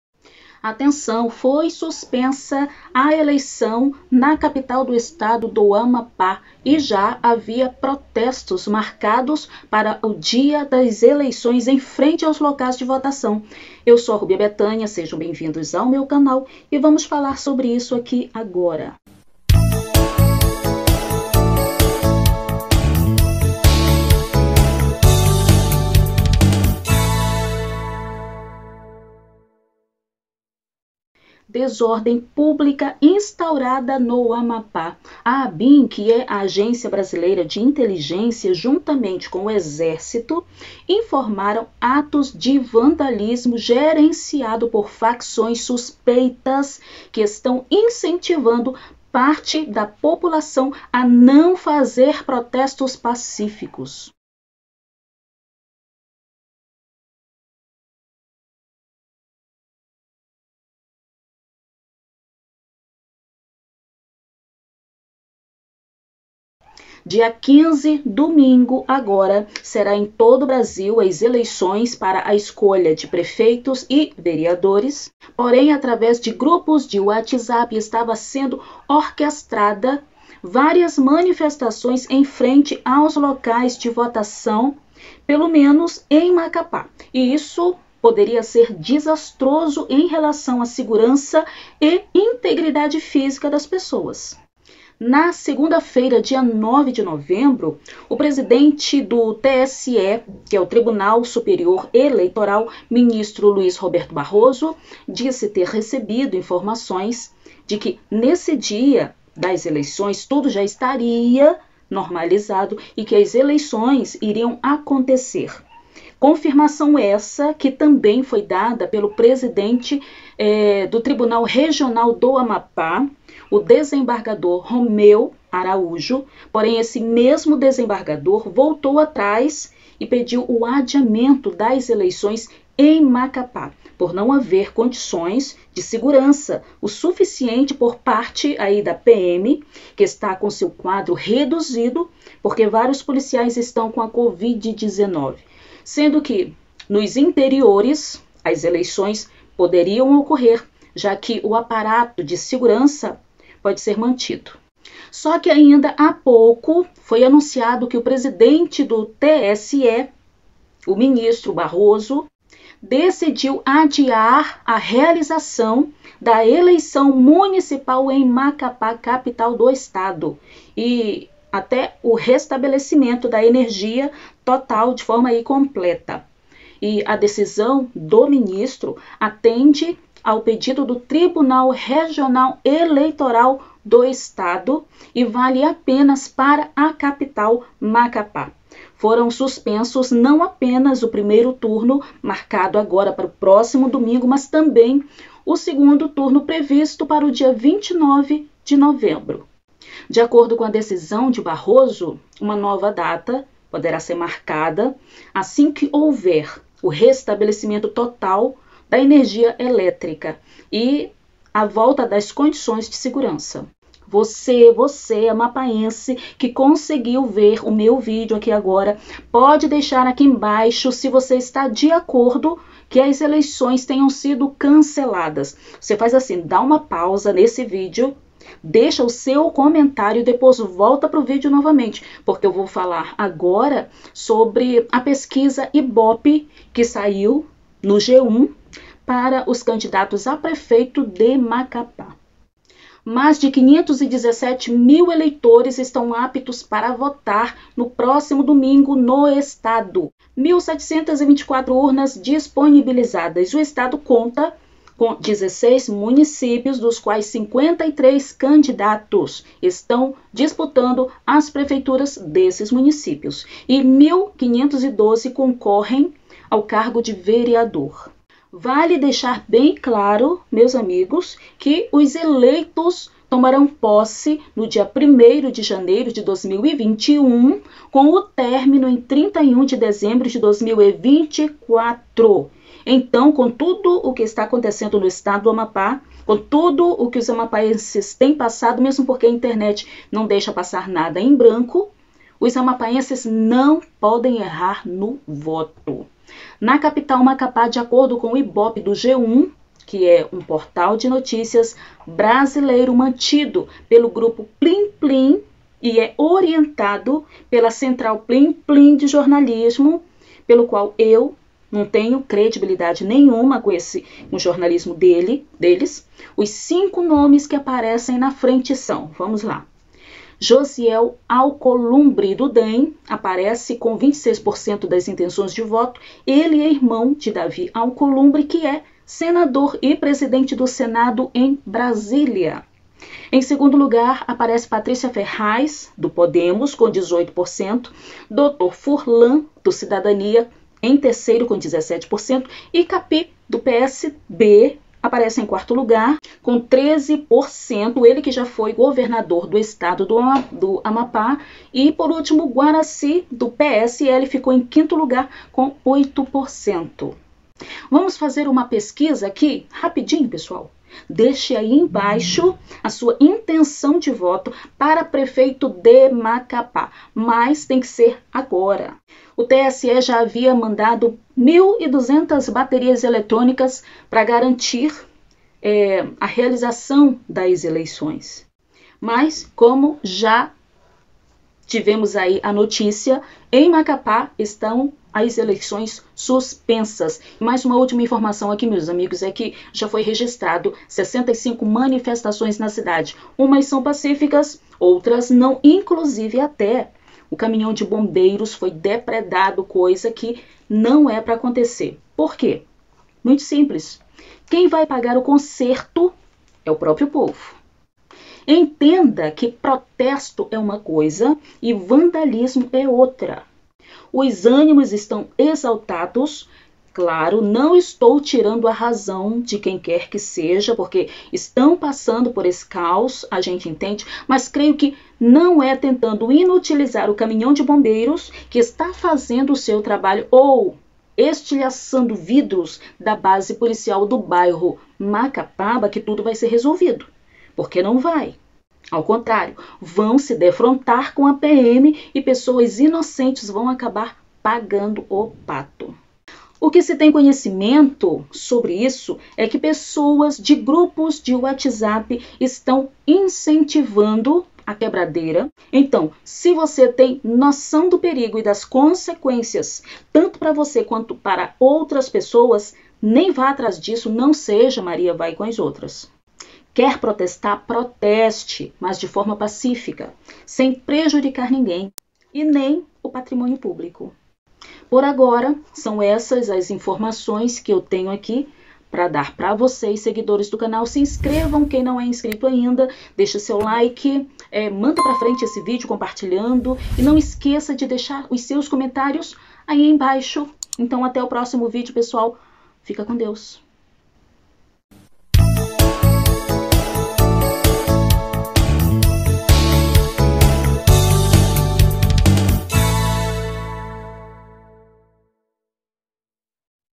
Eleições adiadas em Macapá
TSE suspende eleição municipal em Macapá em meio a apagão
A decisão de Barroso atende a um pedido do Tribunal Regional Eleitoral do Amapá e vale apenas para a capital do Estado. Foram suspensos não apenas o primeiro turno, marcado para o próximo domingo, mas também o segundo, previsto para o dia 29 de novembro.
👉Veja outros videos sobre o caos no Amapá